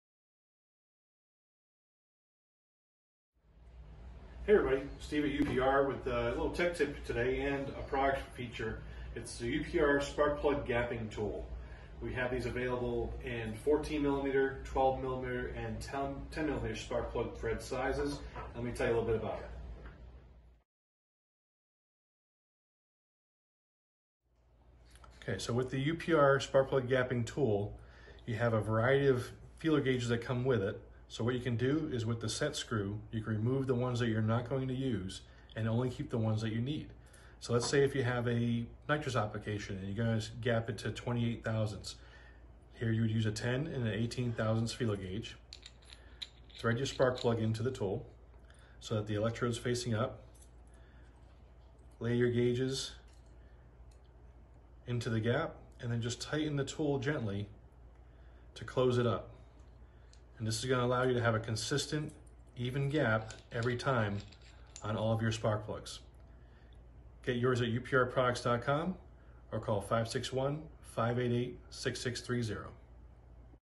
UPR Spark Plug Gapping tool now available!
UPR's Spark Plug Gapping Tool with Built-in Feeler Gauges Designed to save time and give you much more precise gaps. This is what engine builders you as they are telling you how critical your plug gap is. Our new plug gapper will allow you to gap plugs must faster and maintain gap accuracy between all your plugs.